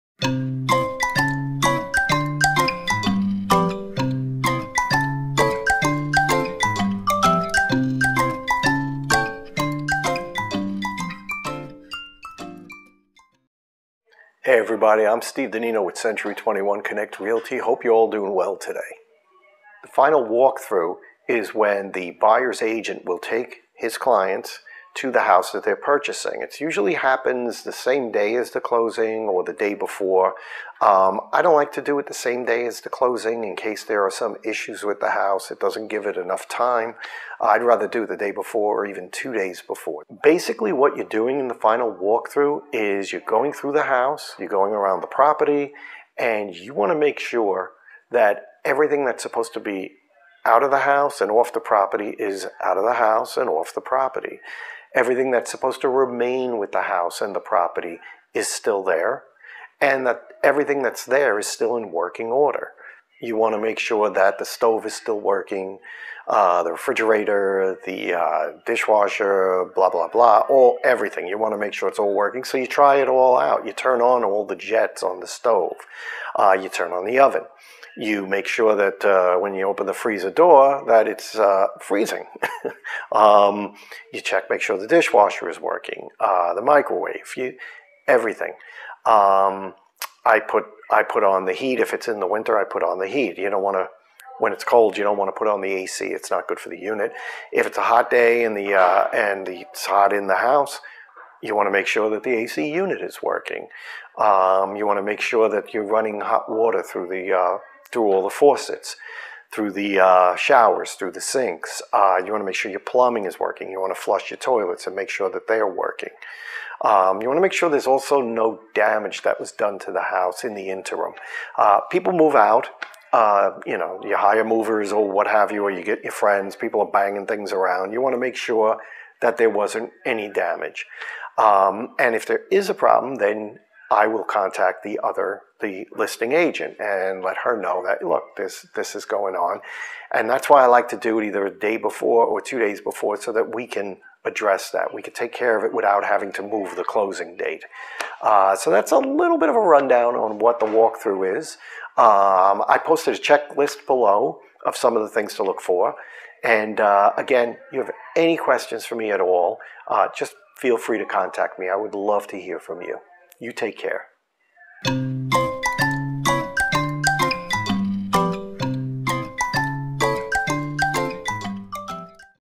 The Walk-Through
Within a day of closing on their new home, it's essential that buyers inspect the home they're about to purchase.

Here are a few things to look for:

- Make sure the appliances work?

- Is the HVAC working?

- Is the plumbing operating normally?

- Did the sellers' remove their personal belongings?

- Were agreed upon repairs completed?

- Was the attic checked?

- Was the basement/crawlspace checked?

- Is the home "broom clean"?